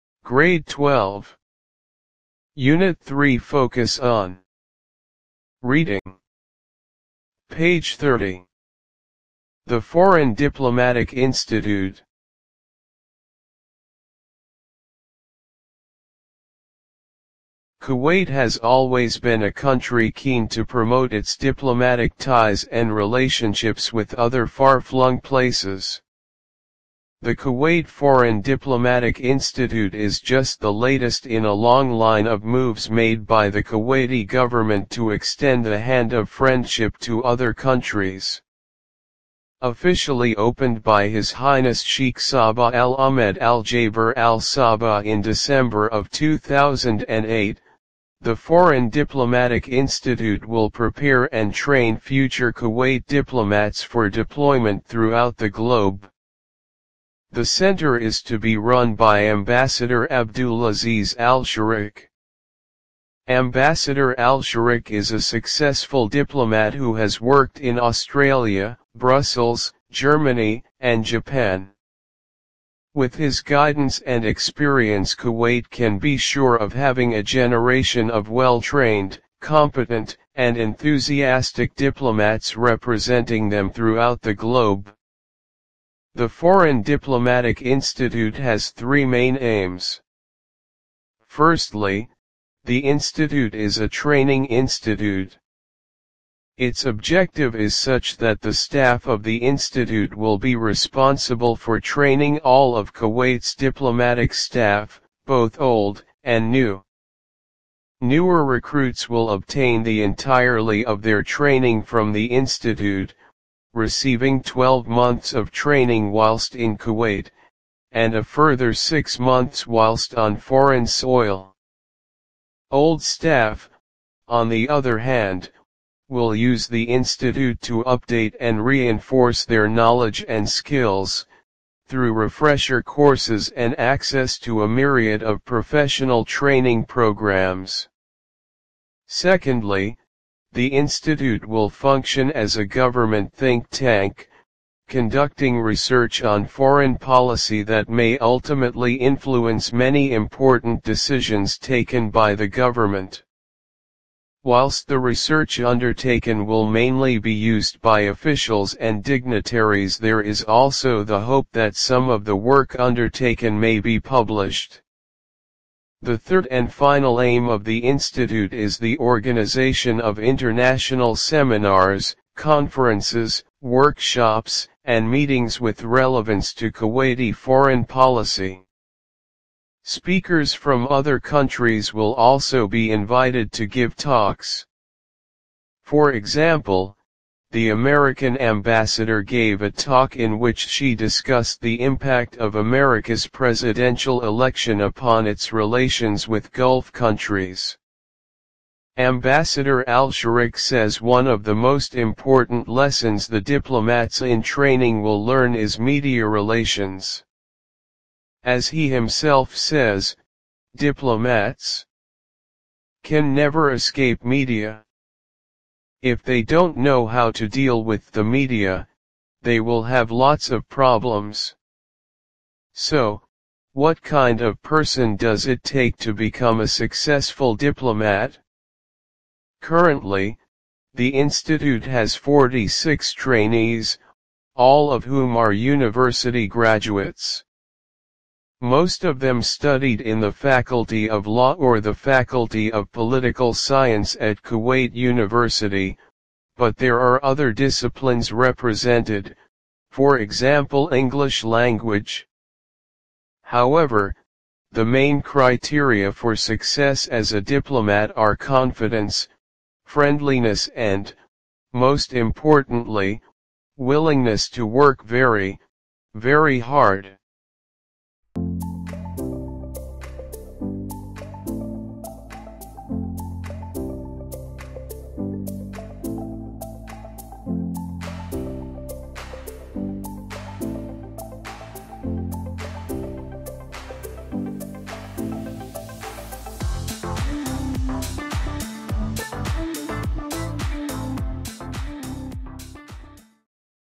G12 U3 P30 "The foreign Diplomatic Institute" (Focus on) قراءة درس / مترجم
Grade 12
Unit 3 Focus on
Reading
Page 30
The foreign Diplomatic Institute.
Kuwait has always been a country keen to promote its diplomatic ties and relationships with other far-flung places. The Kuwait Foreign Diplomatic institute is just the latest in a long line of moves made by the Kuwaiti government to extend the hand of friendship to other countries.
Officially opened by His Highness Sheikh Sabah Al-Ahmed Al-Jaber Al-Sabah in December of 2008, the Foreign Diplomatic Institute will prepare and train future Kuwait diplomats for deployment throughout the globe.
The centre is to be run by Ambassador Abdulaziz Al-Sharikh. Ambassador Al-Sharikh is a successful diplomat who has worked in Australia, Brussels, Germany and Japan. With his guidance and experience Kuwait can be sure of having a generation of well-trained, competent and enthusiastic diplomats representing them throughout the globe.
The Foreign Diplomatic Institute has three main aims. Firstly, the institute is a training institute. Its objective is such that the staff of the institute will be responsible for training all of Kuwait’s diplomatic staff, both old and new. Newer recruits will obtain the entirely of their training from the institute, receiving twelve months of training whilst in Kuwait, and a further six months whilst on foreign soil. Old staff, on the other hand, will use the institute to update and reinforce their knowledge and skills, through refresher courses and access to a myriad of professional training programmes.
Secondly, the institute will function as a government think-tank, conducting research on foreign policy that may ultimately influence many important decisions taken by the government. Whilst the research undertaken will mainly be used by officials and dignitaries there is also the hope that some of the work undertaken may be published.
The third and final aim of the institute is the organization of international seminars, conferences, workshops and meetings with relevance to Kuwaiti foreign policy. Speakers from other countries will also be invited to give talks. For example, the American Ambassador gave a talk in which she discussed the impact of America’s presidential election upon its relations with Gulf countries.
Ambassador Al-Sharikh says one of the most important lessons the diplomats-in-training will learn is media relations. As he himself says, diplomats. Can never escape media. If they don’t know how to deal with the media, they will have lots of problems.
So, what kind of person does it take to become a successful diplomat? Currently, the institute has 46 trainees, all of whom are university graduates. Most of them studied in the Faculty of law or the faculty of political science at Kuwait University, but there are other disciplines represented, for example English Language. However, the main criteria for success as a diplomat are confidence, friendliness and, most importantly, willingness to work very, very hard.